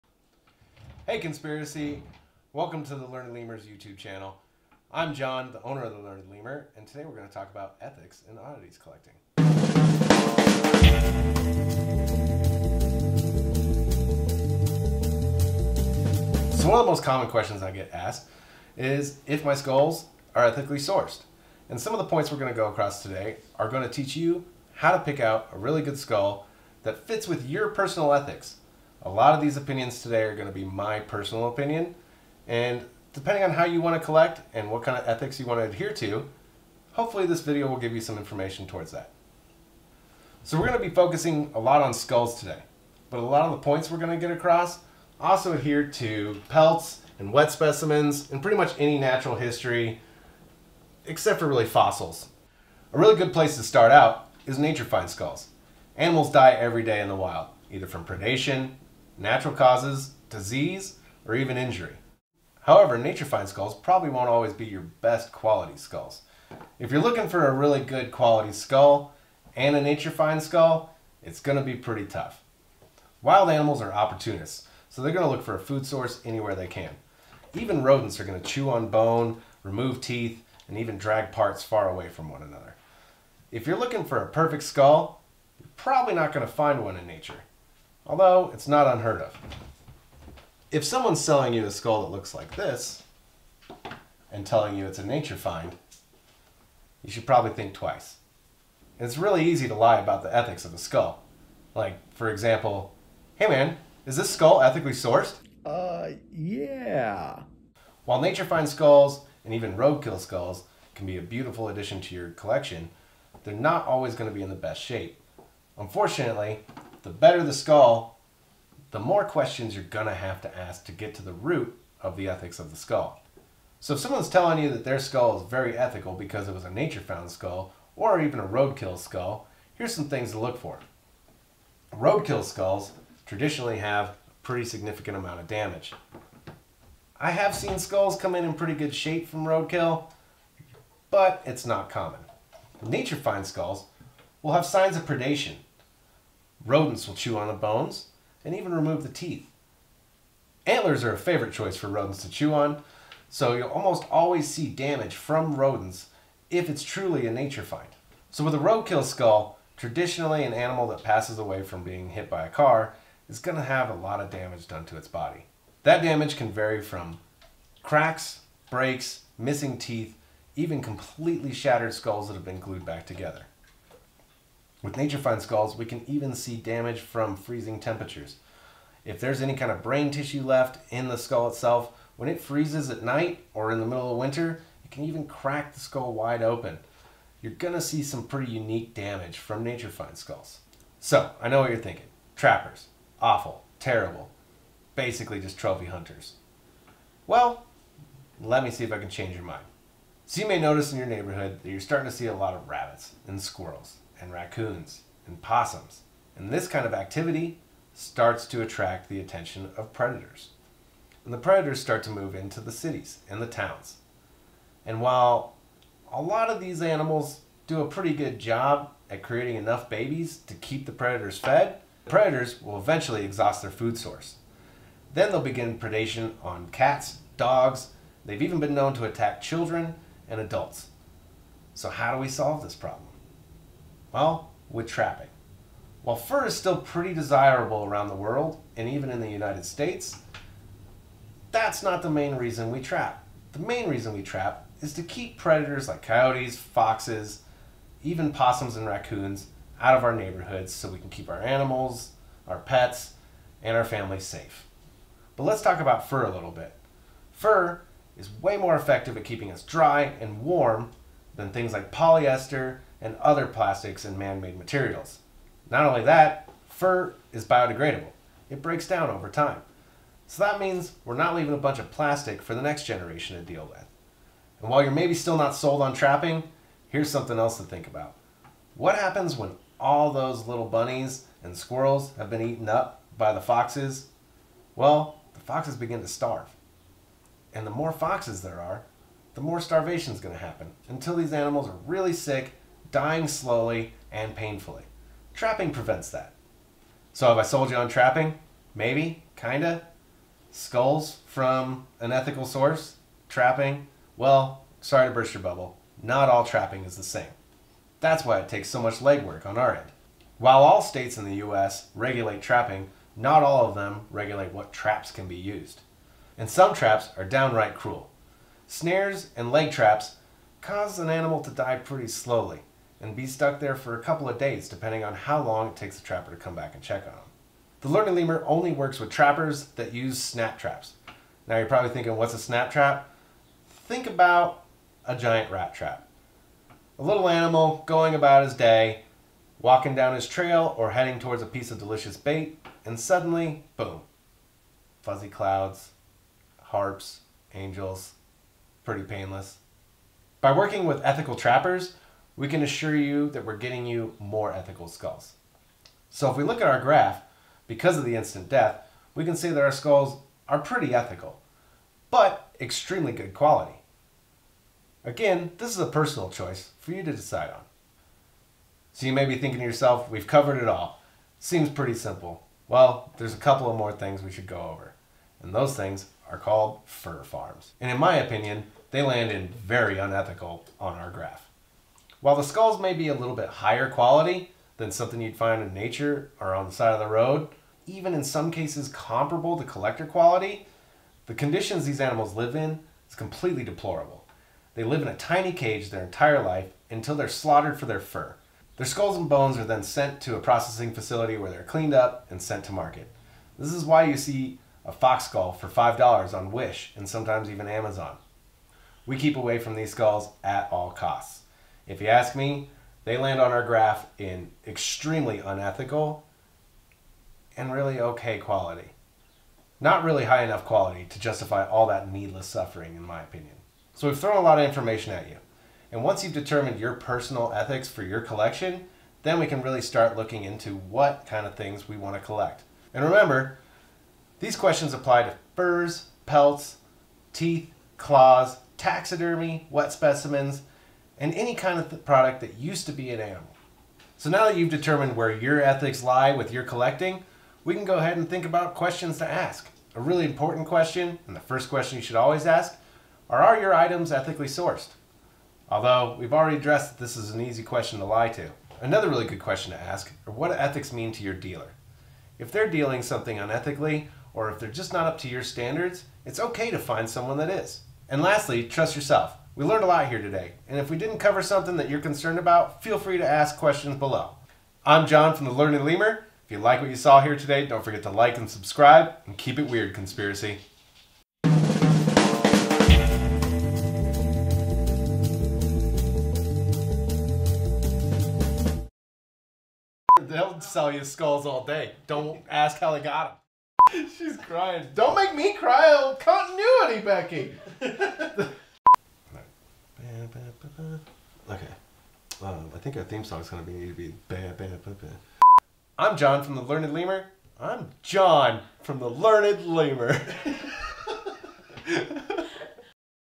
Ethics in Oddities | The Learned Lemur
Buying oddities can be tricky even before you consider the ethics of where they came from. Let's talk about some things you should know and consider when buying your oddities. And don't forget to stick around for the outtakes at the end!